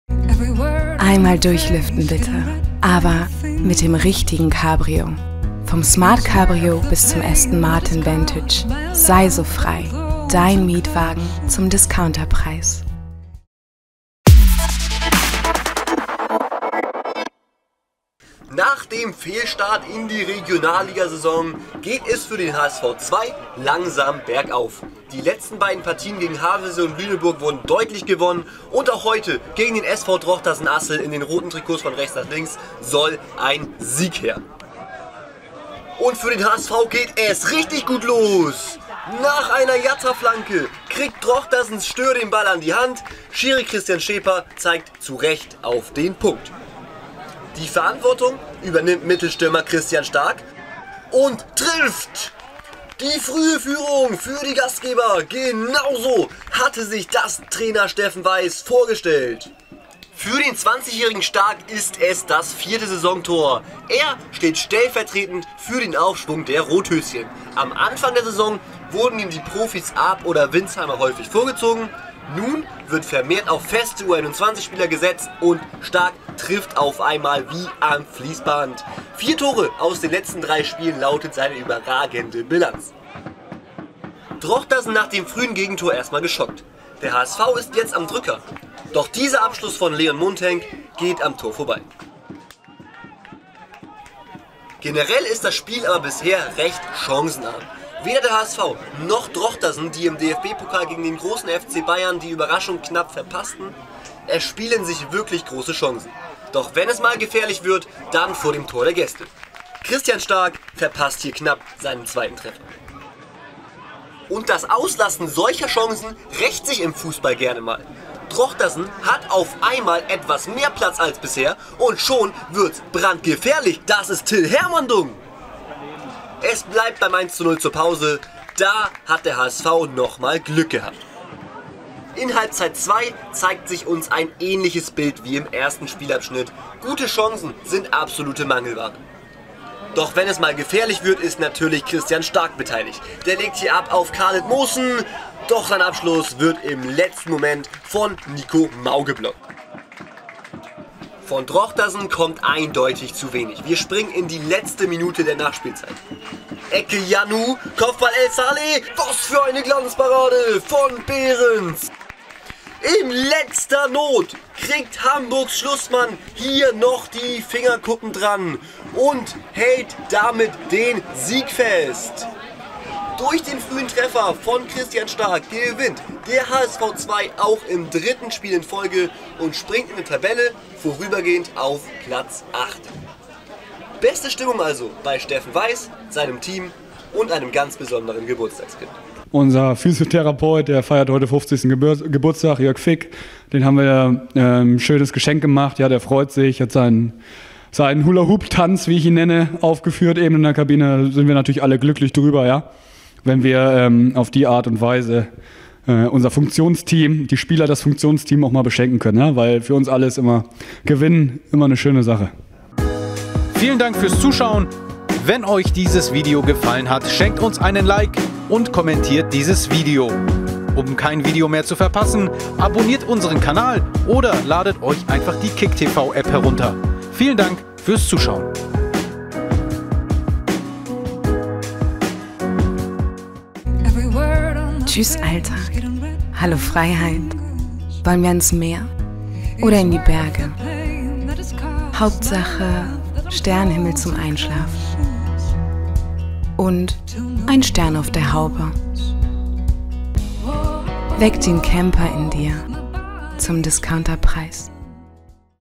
Hamburger SV II - SV Drochtersen/Assel (11. Spieltag, Regionalliga Nord)
Rothösschen-Siegesserie hält an!

11. Spieltag (21.09.18)

Endergbnis: 1:0 (1:0)

Tore:
1:0 Christian Stark

Aufstellung Hamburger SV II:
1 Morten Behrens, 4 Jonas David, 7 Fabian Gmeiner, 9 Christian Stark, 10 Khaled Mohssen, 11 Moritz Kwarteng, 14 Jonas Behounek, 15 Mats Köhlert, 16 Bakery Jatta, 19 Leon Mundhenk, 23 Maximilian Geißen

Ersatzspieler:
12 Jakob Golz,8 Kusi Kwame, 13 Niklas Schneider, 17 Justin Huber, 18 Marco Drwaz, 24 Aaron Opoku, 26 Dominic Cyriacks

Trainer:
Steffen Weiß

Aufstellung SV Drochtersen/Assel:
30 Patrick Siefkes, 3 Meikel Klee, 8 Marcel Andijanic, 10 Florian Nagel, 11 Jasper Gooßen, 12 Nico Mau, 13 Sören Behrmann, 14 Alexander Neumann, 20 Oliver Iannou, 23 Julian Stöhr, 24 Till Hermandung

Ersatzspieler:
1 Fabian Klinkmann, 5 Eyke Kleine, 9 Jan-Ove Edeling, 21 Dimitri Fiks, 22 Hassan El Saleh, 28 Liam Giwah, 77 Sung Hyun Jung

Trainer:
Lars Uder

Produzent:
Jörn Duddeck

Kommentar:
Manuel Meyer